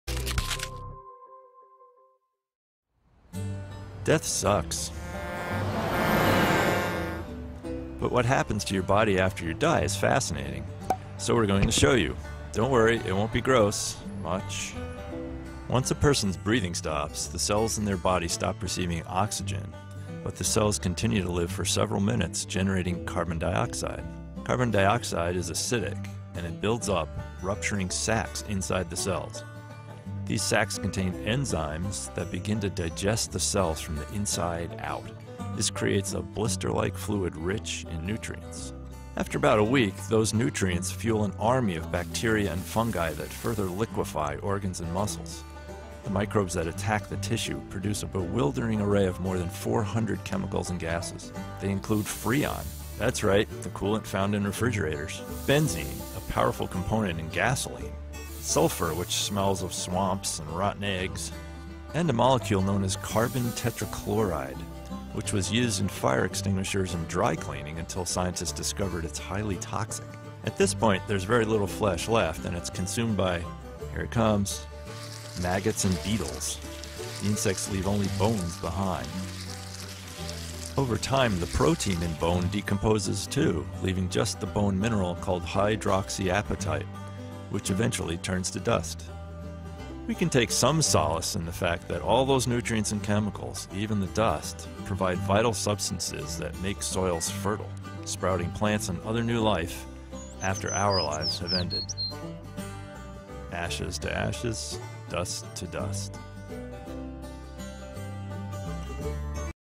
Öldükten Sonra Toprağın Altında Bedeninize Ne Oluyor ?
Ölürüz, toprağa gömülürüz ve doğal çevrimde yeni bir süreci başlatmış oluruz. Öldükten sonra vücudunuzda olanlar büyüleyicidir.
Paylaşım ve yükleme yaparak bu kanala destek olun.

Facebook.com/akselnuredin
İnstagram.com/akselnuredin
Twitter.com/akselnuredin
Youtube.com/akselnuredin